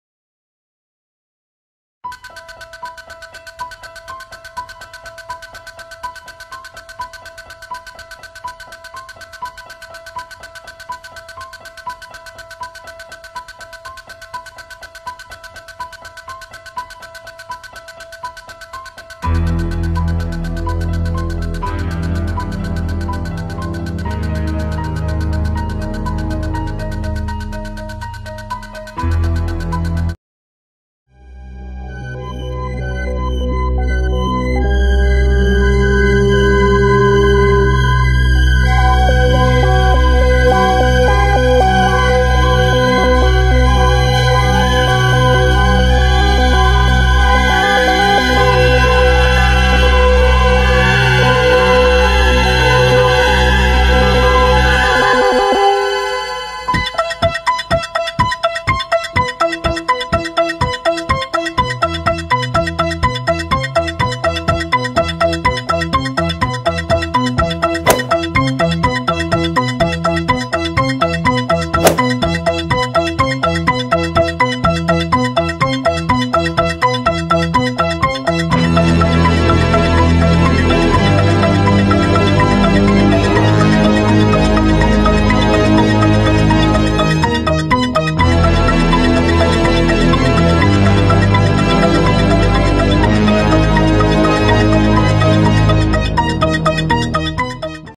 H1 Michael Myers vs. H2 Michael Myers
Just a fun video my friend and I decided to put together. This is basically just another costume test,  not meant to be taken seriously lol.